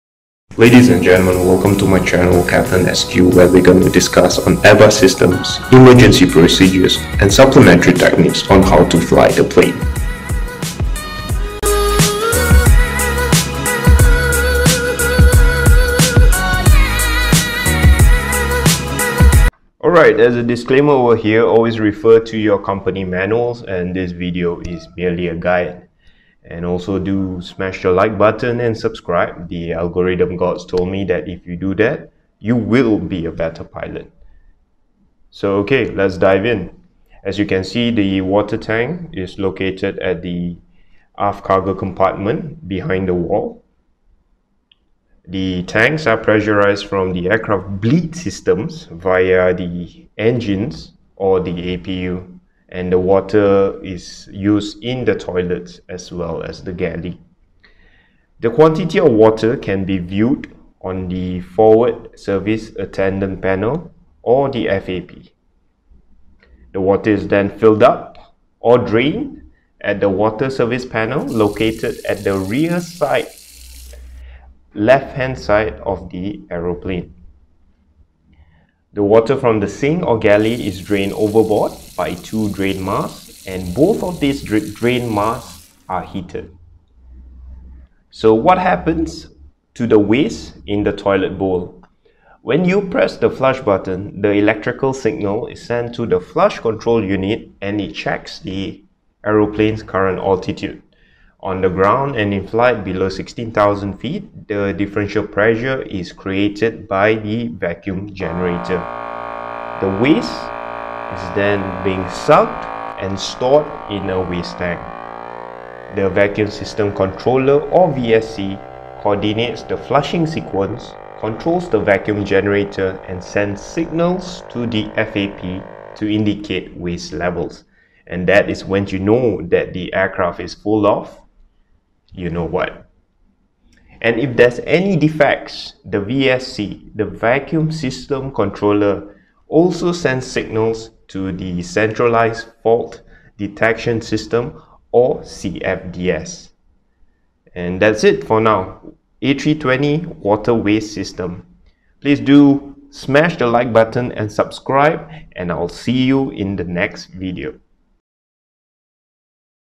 How does A320 water waste system works (MADE EASY)
In this video, you will know how the Airbus A320 water waste system works.